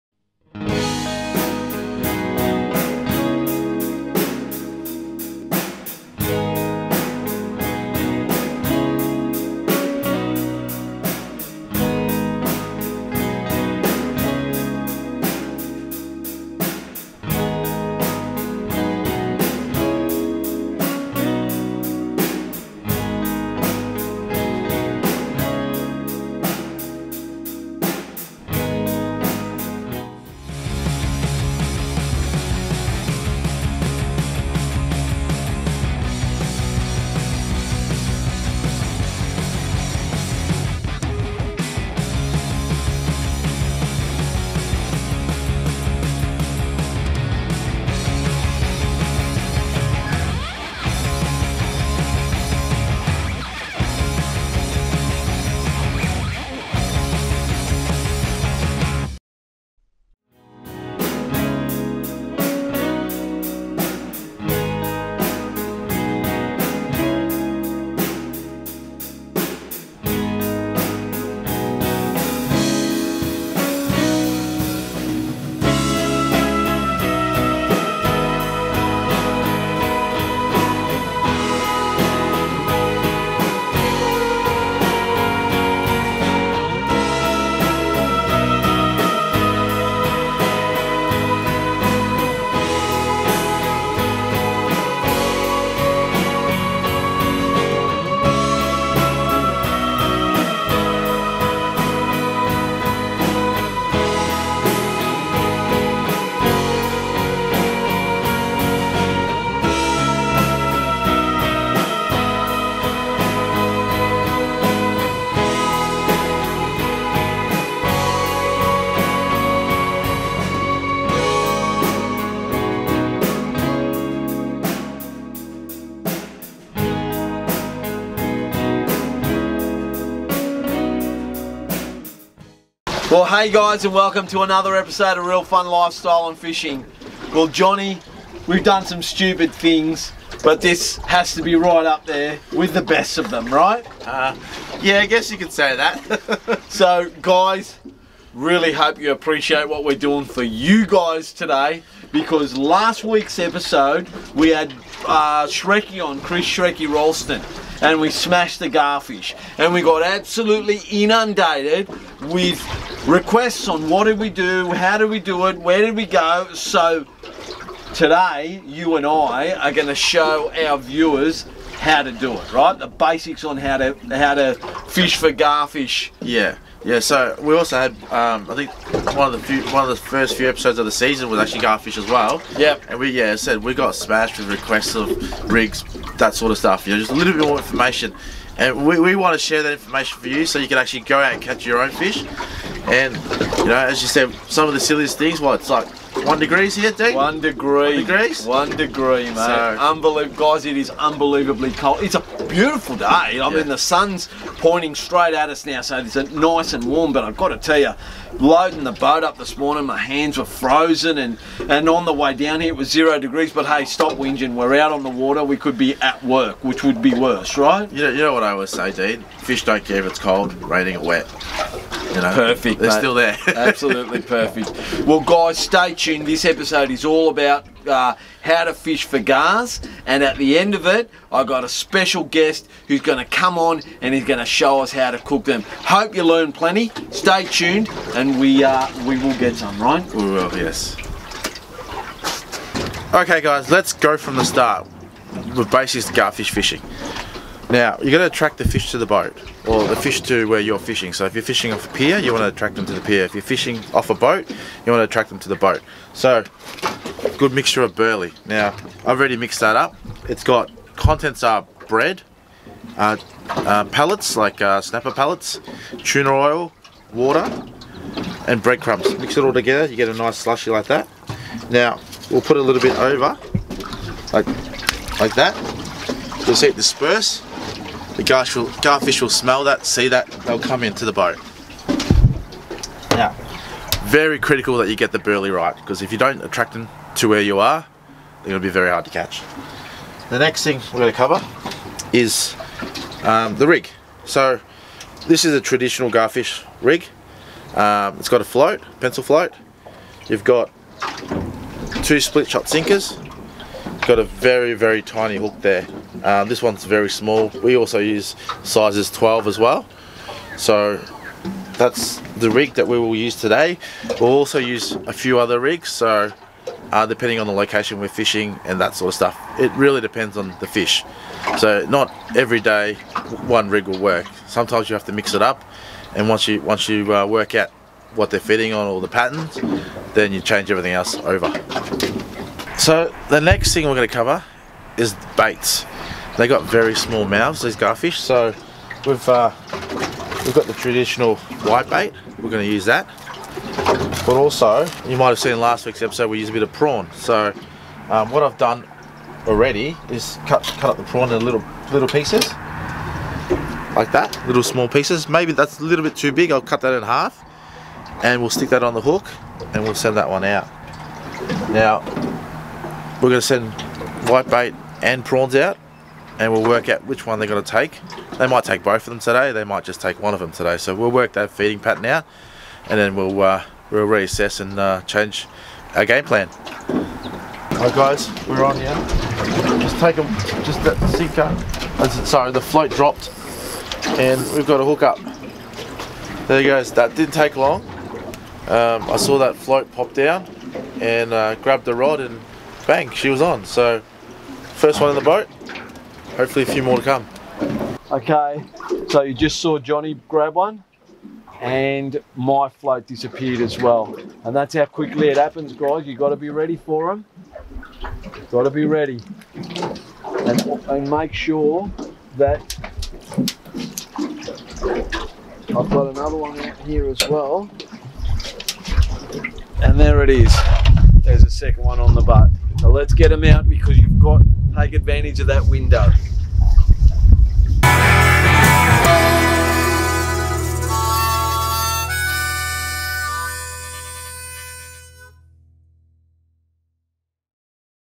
RFLFSER02EP12PT01
Dean And Jony head out to Port Phillip Bay to teach the basics of how to have a successful Garfish Trip. In this Episode the guys are also joined by Theo Rozakis who shows us how to best prepare these underrated fish for the table